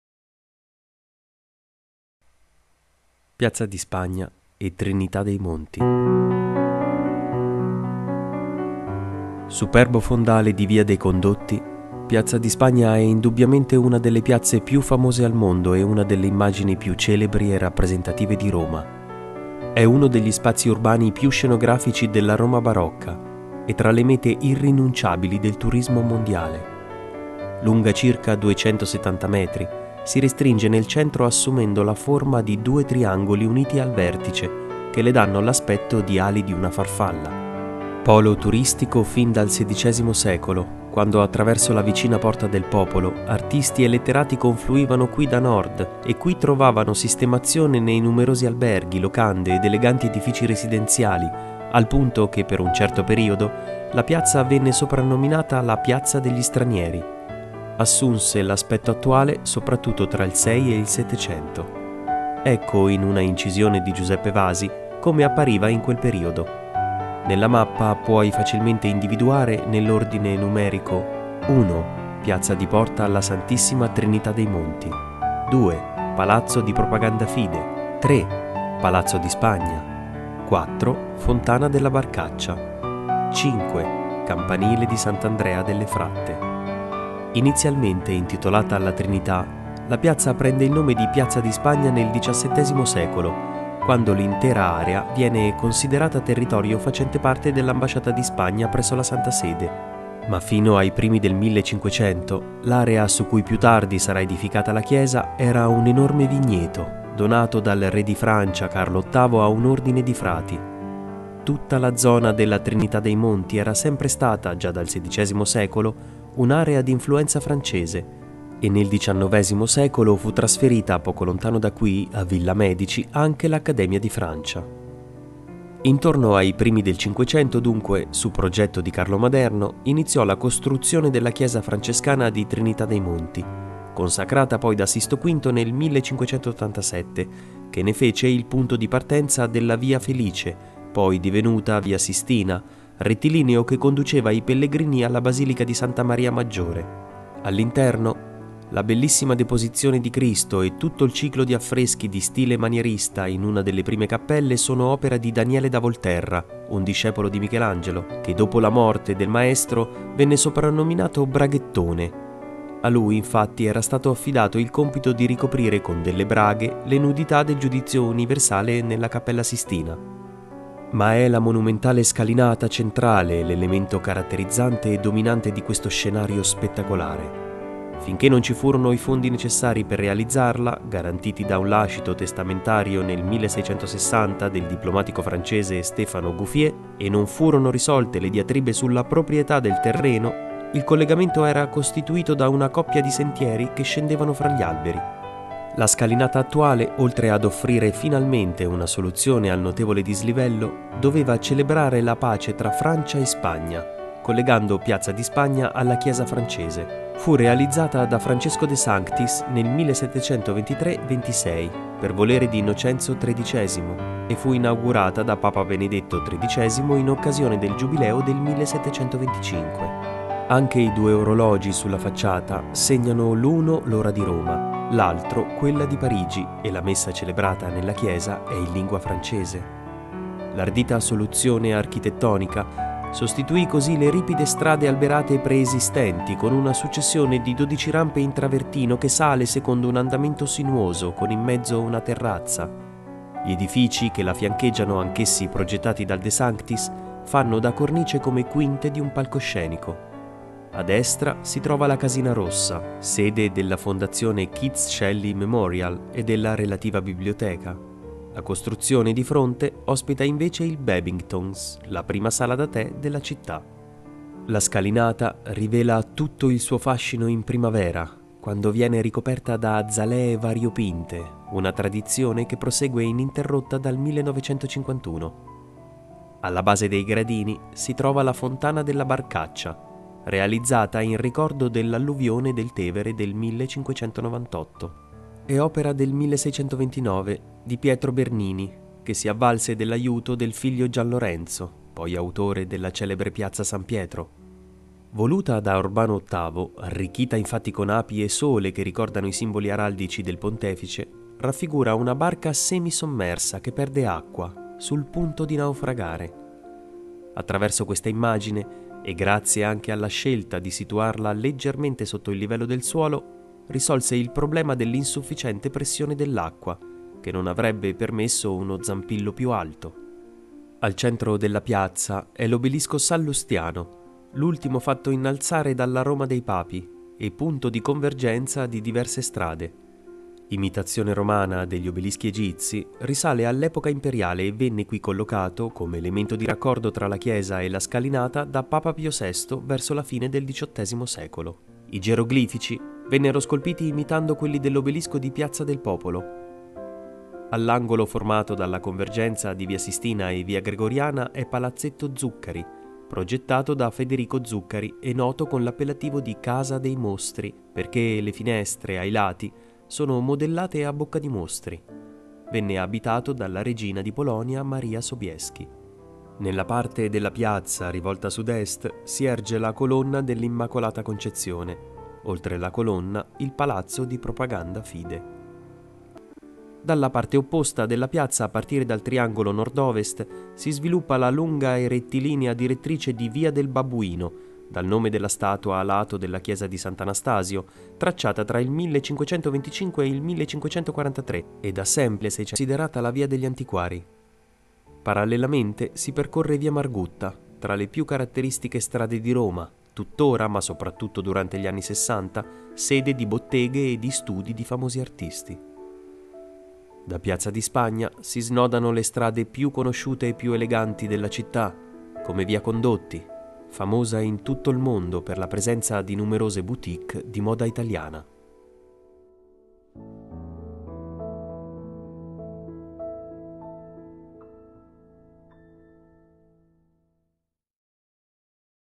PIAZZA DI SPAGNA e Trinità dei Monti, Roma (by Rhome86)
nuovo capitolo della videoguida da me realizzata per gli ospiti del b&b, dedicata alla "città eterna": superbo fondale di via dei Condotti, piazza di Spagna rappresenta uno degli scenari più suggestivi e più celebri di Roma, famosa e ammirata in tutto il mondo